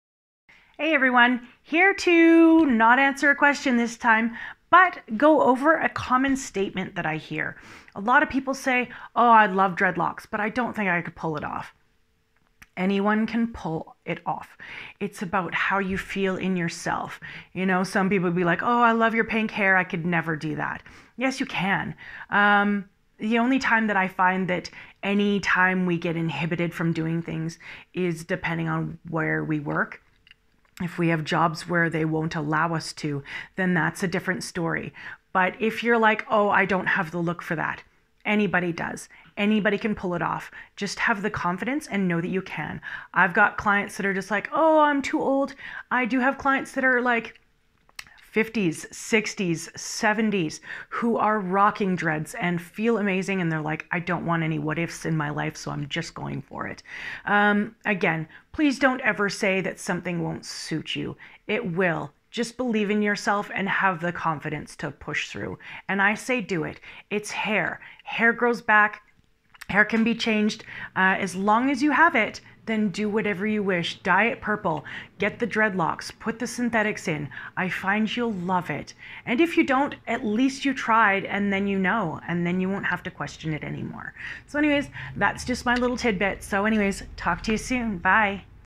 I don't think dreads will suit me... not true!!
mini informational video about rocking those dreads, colored hair, braids ect